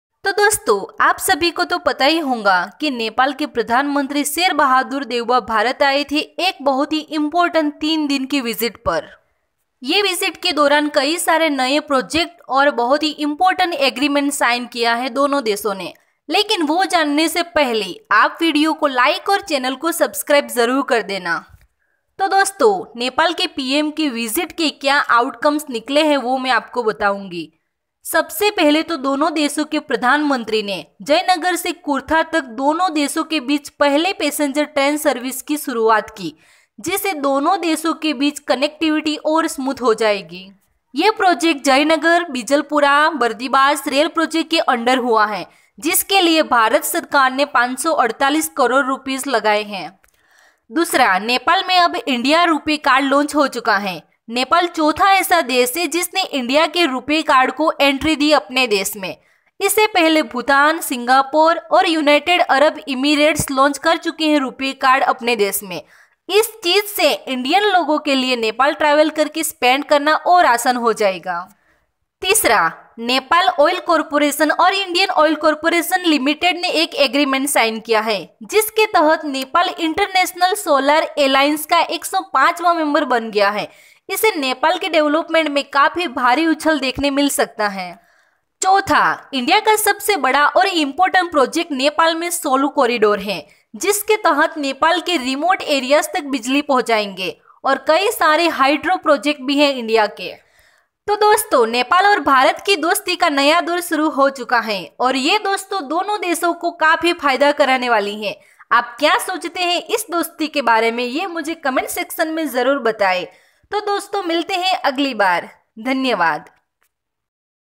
NEPAL PM VISIT| NEPAL- INDIA FRIENDSHIP|NEPAL DEVLOPMENT
NEPAL PM Sher Bahadur deuba 3 day important visit.